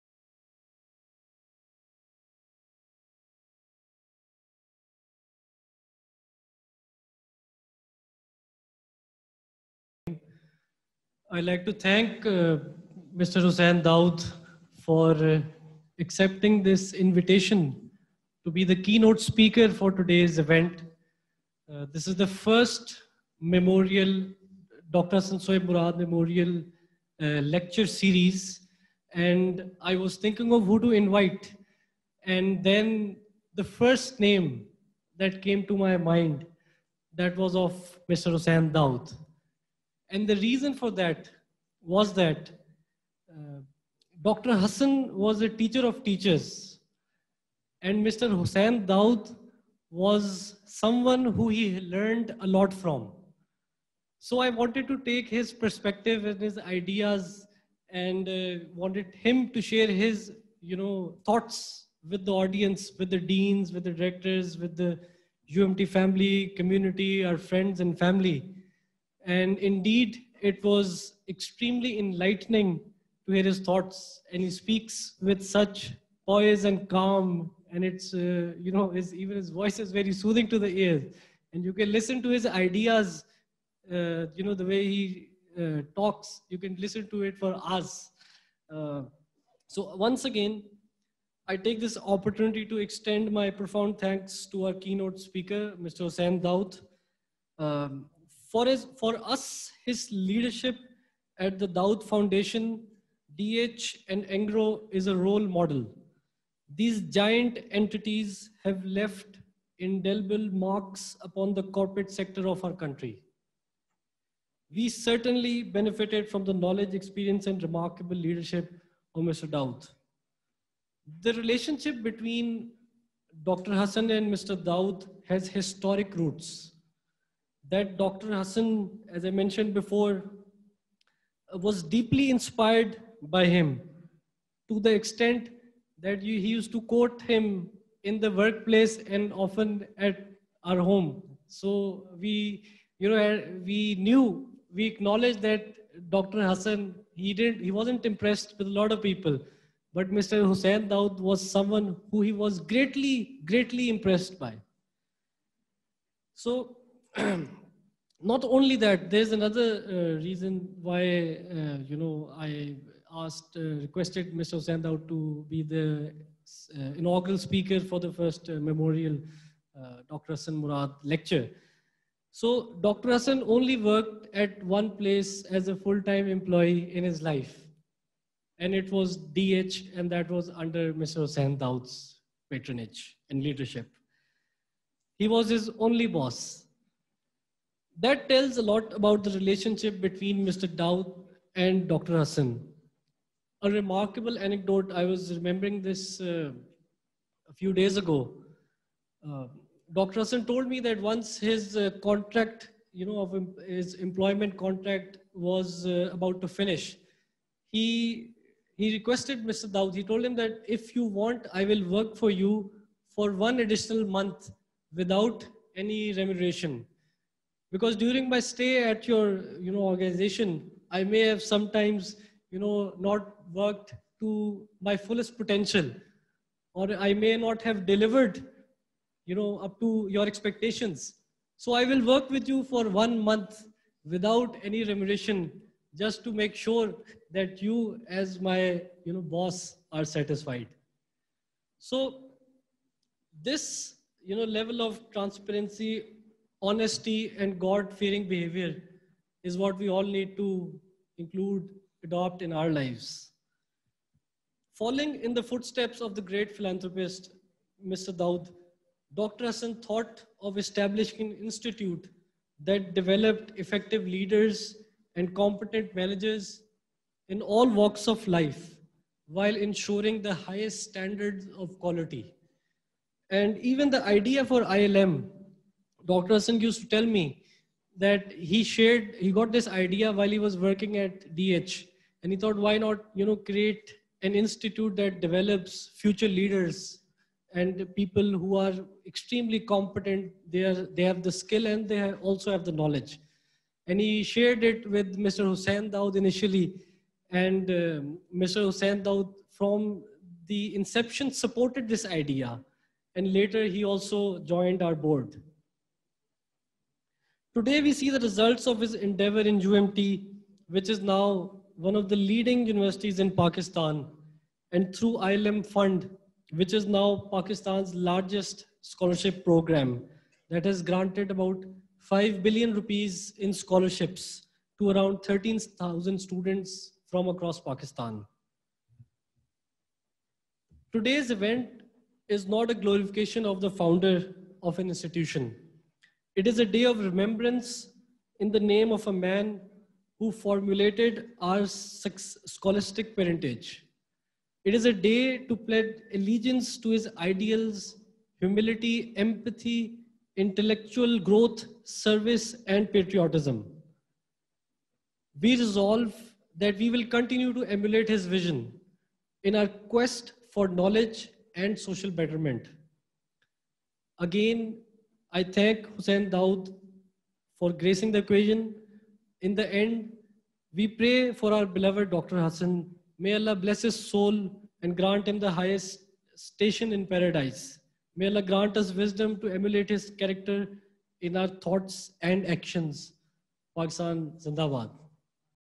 UMT | Ibrahim Hasan Murad Concludes 1st HSM (Shaheed) Memorial Lecture | UMT News
UMT president Ibrahim Hasan Murad Concludes 1st HSM (Shaheed) Memorial Lecture  UMT News
Ibrahim Hasan Murad, President UMT concluded Dr Hasan Sohaib Murad (Shaheed) memorial lecture with a vote of thanks. He extended immense gratitude towards Hussain Dawood for gracing the event and taking out time from his busy schedule.